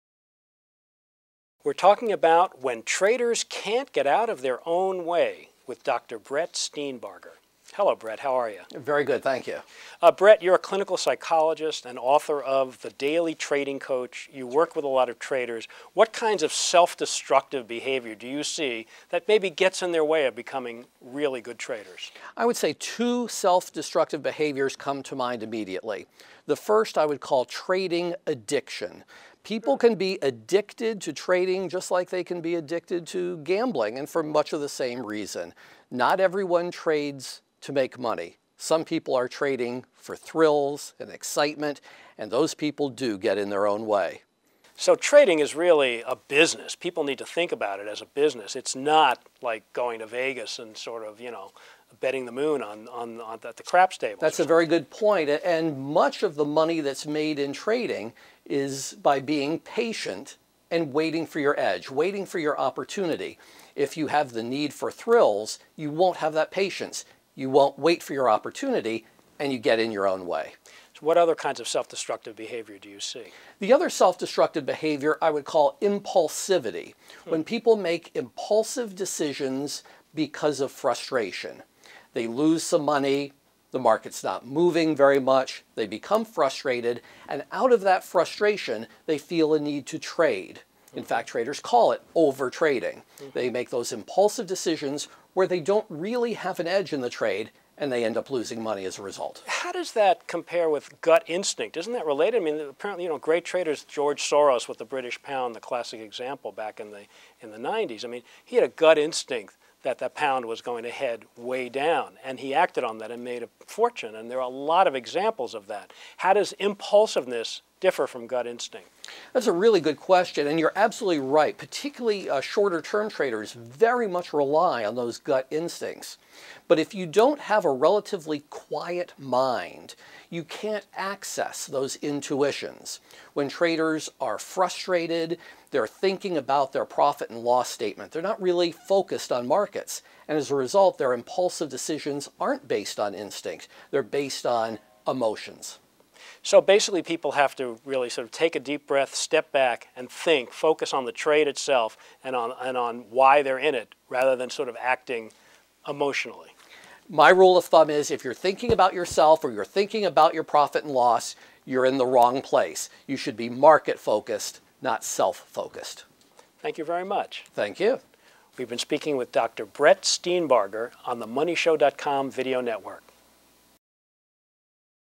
Investors and Traders: Avoid Self-Sabotage in the Markets
Clinical psychologist Brett Steenbarger, who works with many traders and hedge funds, discusses two common self-destructive behaviors that sabotage traders.